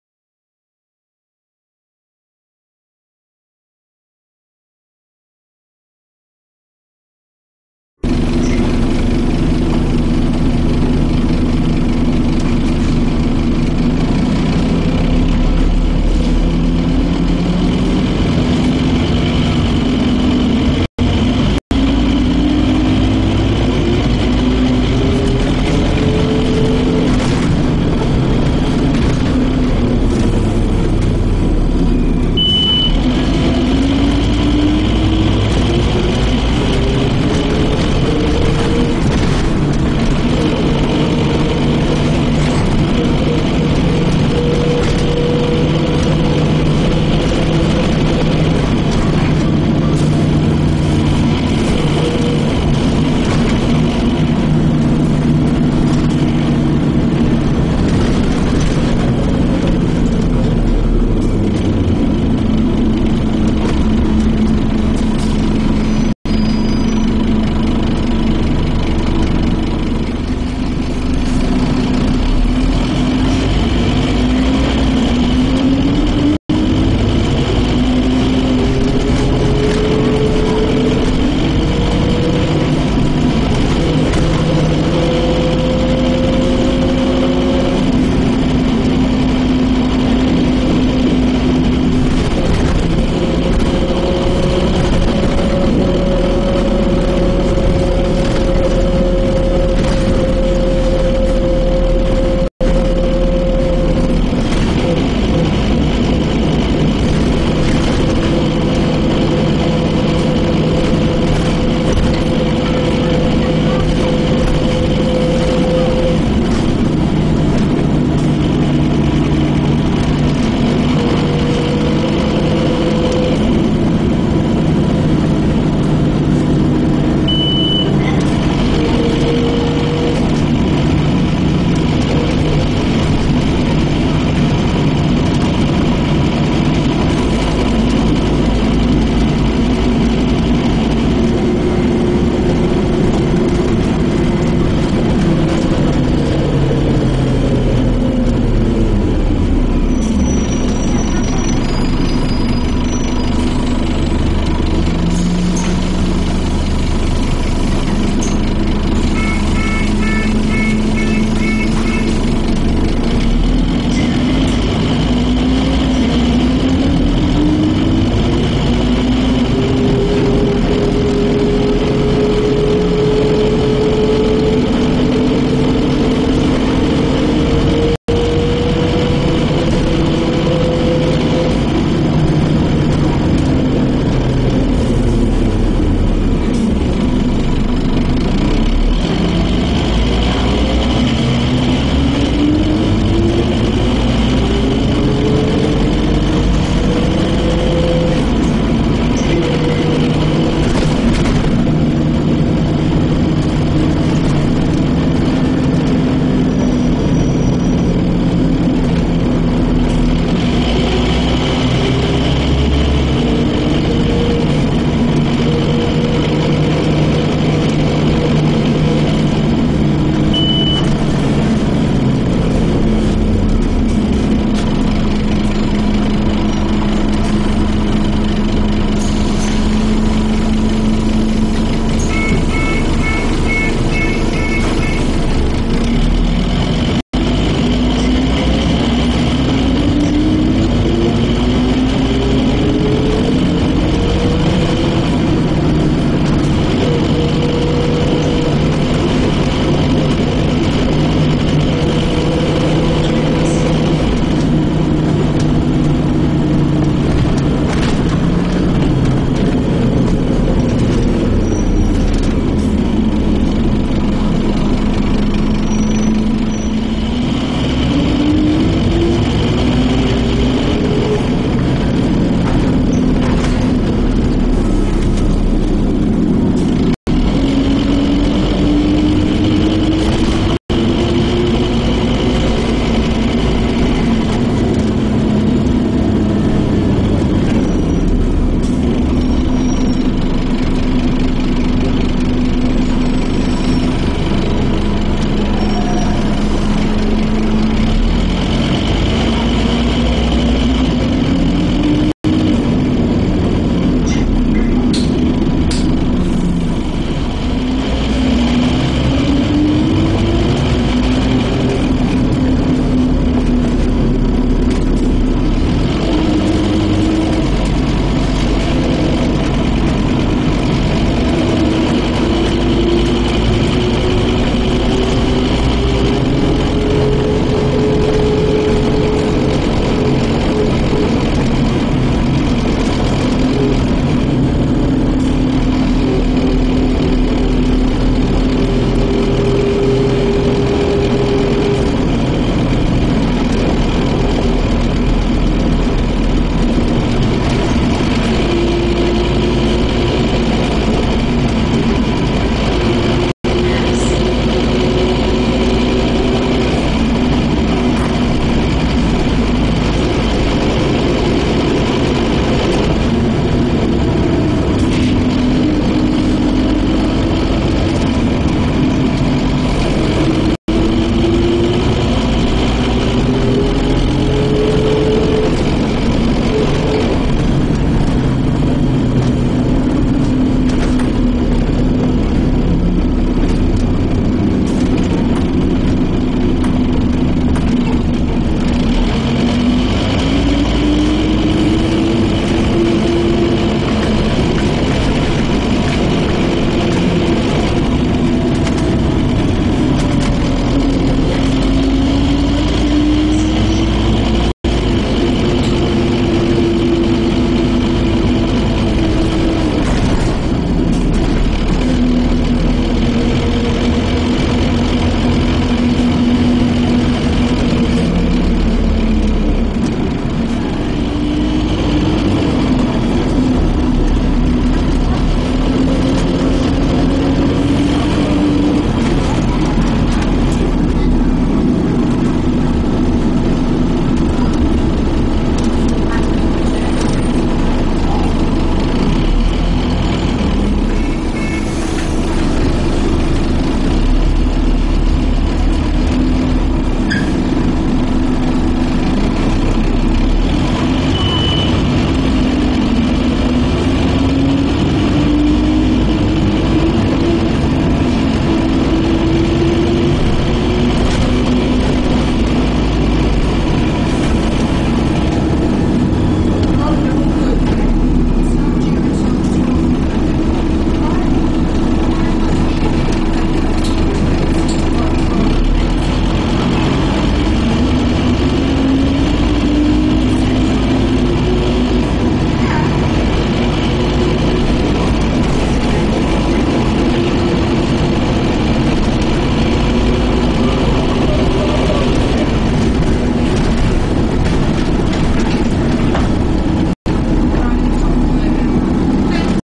Journey on The 281 SP40066 YT09BJX
This bus was serving Route 281 To Tolworth
Route 281: Hounslow Bus Station - Tolworth
Operated by London United RATP Group
 SP40066 YT09BJX